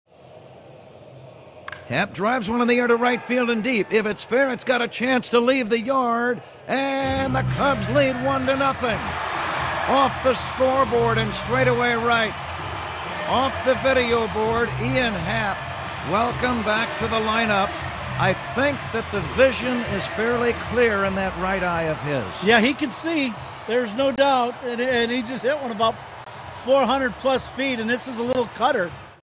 Ian Happ's Leadoff Homer
Happ blasts a leadoff homer to right